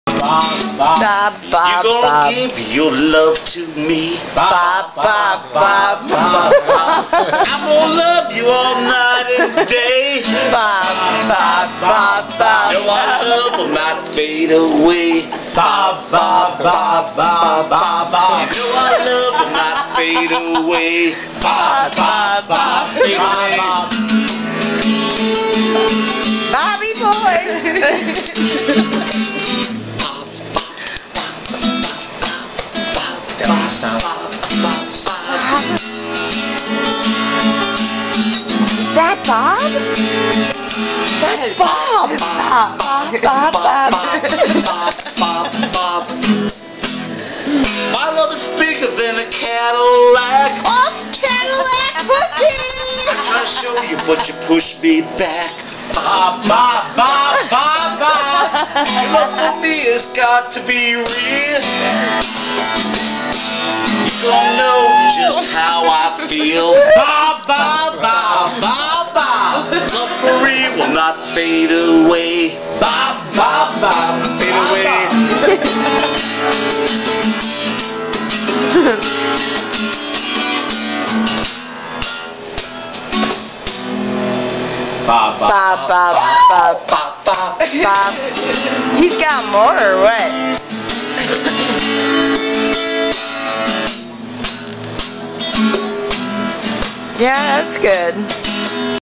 bob song by bob.mov
This is the second bar we stopped at on the way back to wausau after seeing the paulding light. Note: the bar was dead it was midnight and there was only four of us in the bar. Bob Rocks!